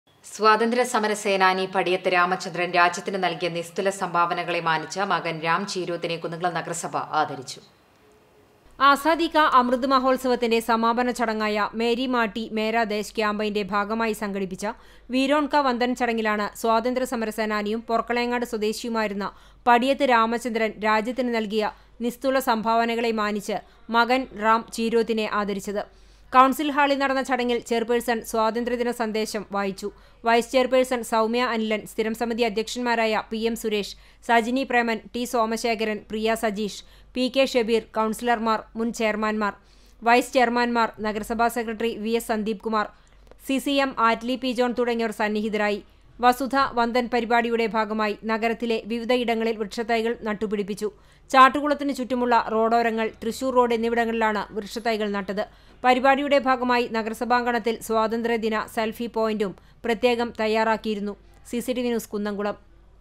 സ്വാതന്ത്ര്യ സമര സേനാനി പടിയത്ത് രാമചന്ദ്രന്റെ മകന്‍ രാം ചീരോത്തിനെ കുന്നംകുളം നഗരസഭ ആദരിച്ചു.
സ്വാതന്ത്ര്യ സമര സേനാനി പടിയത്ത് രാമചന്ദ്രന്‍ രാജ്യത്തിനു നല്‍കിയ നിസ്തുല സംഭാവനകളെമാനിച്ച് മകന്‍ രാം ചീരോത്തിനെ കുന്നംകുളം നഗരസഭ  ആദരിച്ചു.